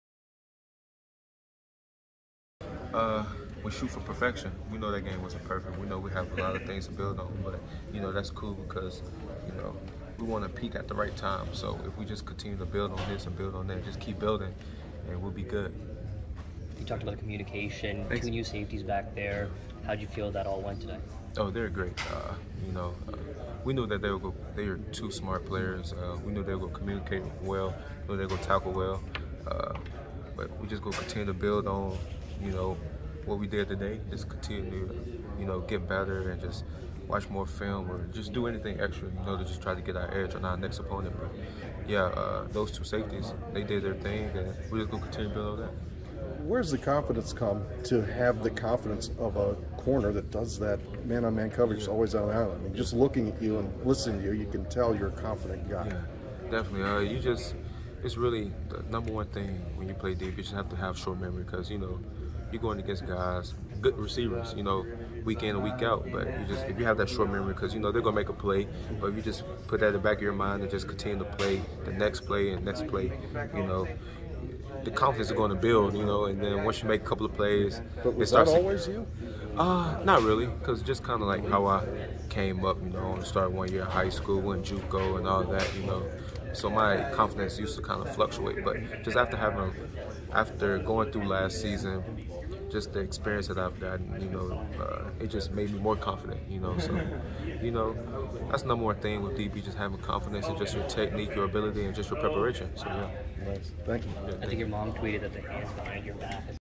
Jakorian Bennett after Buffalo - Maryland Football
Terp cornerback Jakorian Bennett, from Mobile, AL and Hutchinson CC (Kansas) talks about the confidence you need to play cornerback